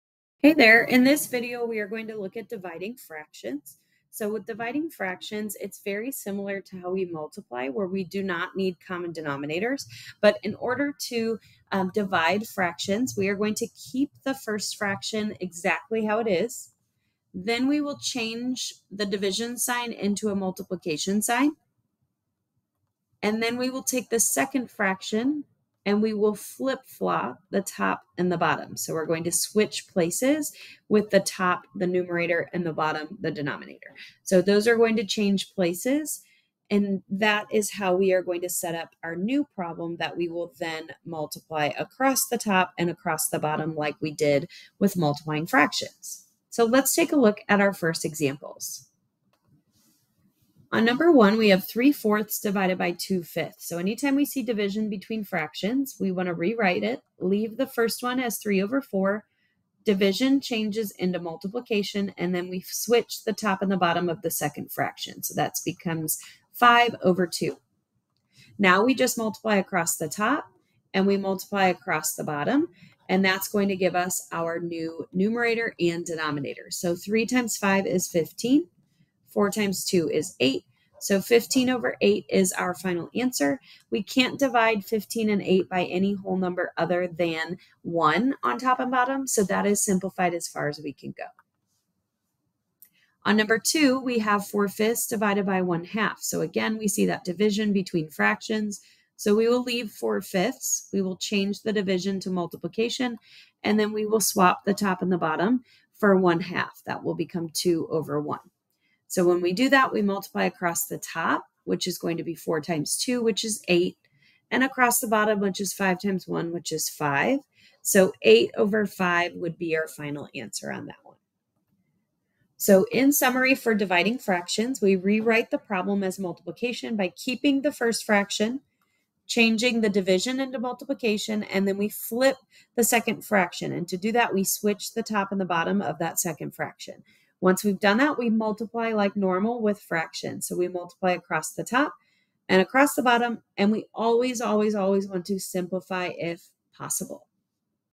Division of Fractions - iPREP's Basic Math Video Lessons
Learn how to divide fractions with ease using our detailed guide! This video provides step-by-step instructions and examples to help you confidently handle fraction division.

*Division of Fractions:* Understand that dividing two fractions is the same as multiplying the first fraction by the inverse of the second fraction. Simply flip the numerator and denominator of the second fraction and change the division sign to multiplication. Example: 4/5 ÷ 1/2 = 4/5 x 2/1 = 8/5.

This technique will enhance your skills in dividing fractions, making complex fraction problems more manageable.

Watch now to improve your understanding of fraction division and boost your mathematical confidence!